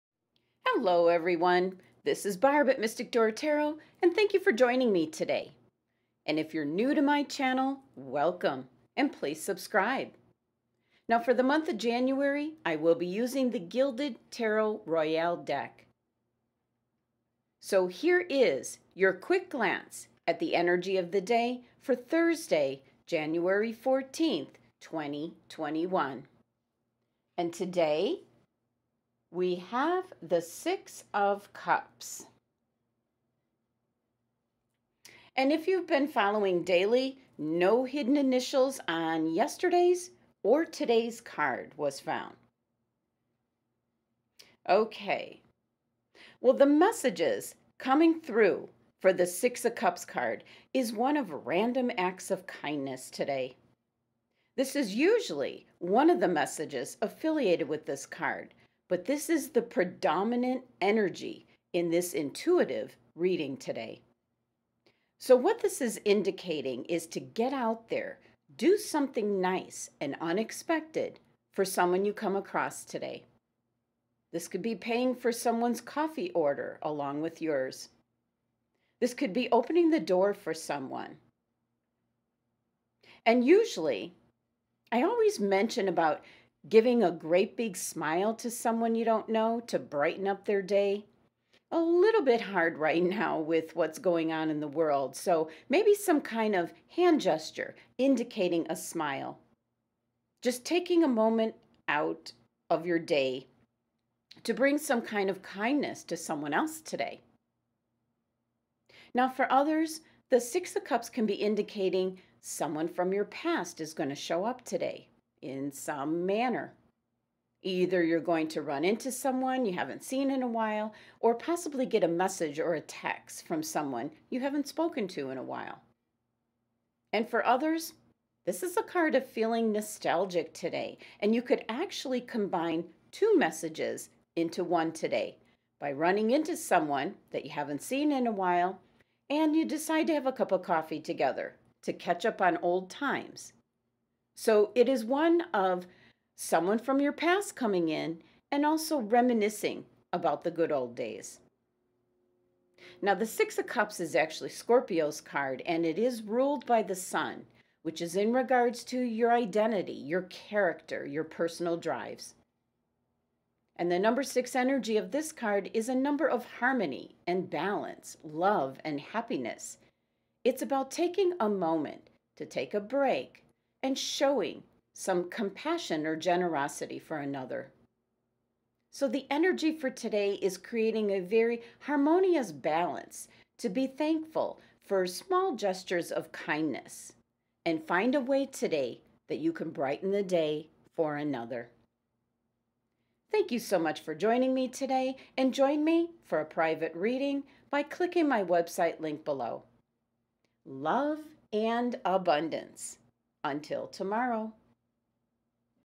Daily Tarot Reading ~ Jan 14, 2021 ~ Mystic Door Tarot ~ DAILY QUICK GLANCE!
Daily Tarot Reading for Jan 14, 2021 by Mystic Door Tarot
Offering intuitive general daily tarot, weekly tarot, and monthly tarot readings
TODAY ~ 6 OF CUPS
This is your quick glance reading for the energy of the day ahead!

Gilded Tarot Royale Deck - Ciro Marchetti & Barbara Moore